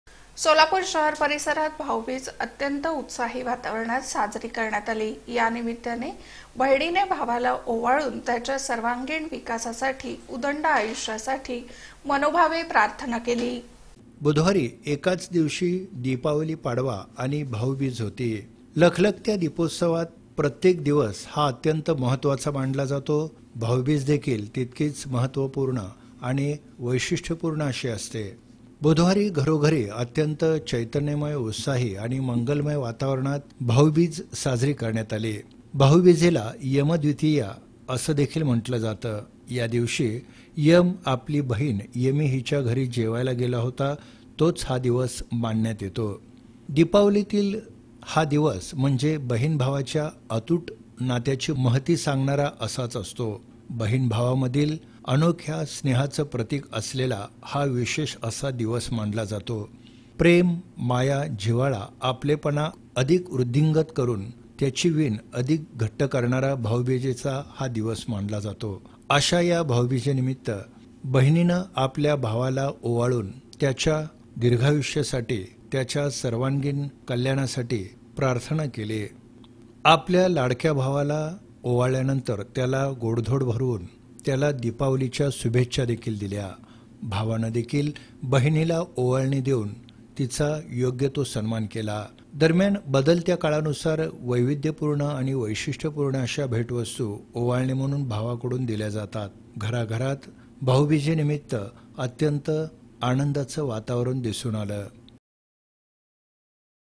बहिणीने केले भावाचे औक्षण... भावाच्या सर्वांगीण कल्याणासाठी प्रार्थना... | BHAUBEEJ
बहिणीने केले भावाचे औक्षण... भावाच्या सर्वांगीण कल्याणासाठी प्रार्थना...